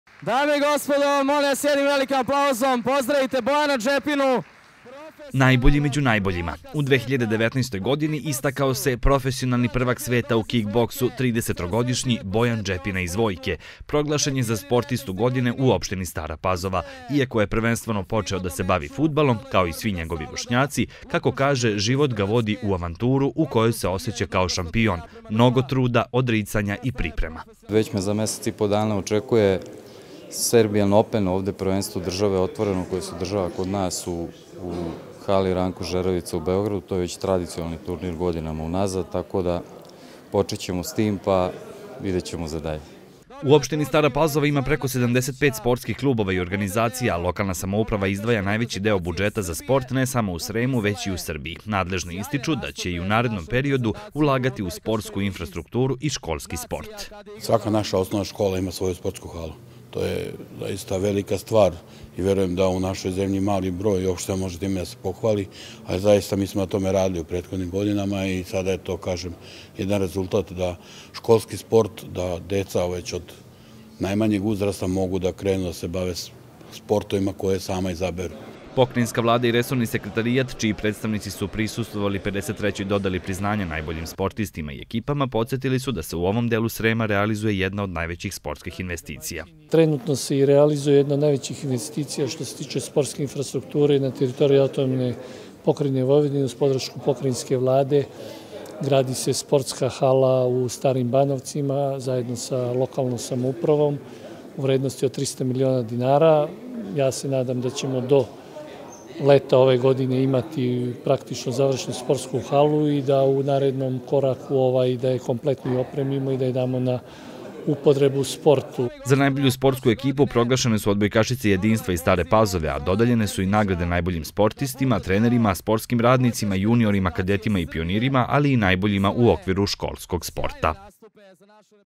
Bojan Džepina - Najbolji sportista za 2019.
Najbolji sportista u opštini Stara Pzaova za 2019. godinu je Bojan Džepina iz Vojke, dok je za priznanje za najbolju ekipu pripalo odbojkašicama “Jedinstva” iz Stare Pazove. Na 53. Proglašenju najboljih dodeljenu su nagrade najboljim sportistima, sportskim ekipama, trenerima, sportskim radnicima, juniorima, kadetima i pionirima, ali i najboljima u okviru školskog sporta. Organizator ove sportske manifestacije je Optina Stara Pazova u saradnji sa Sportskim savezom opštine Stara Pazova i RTV Stara Pazova.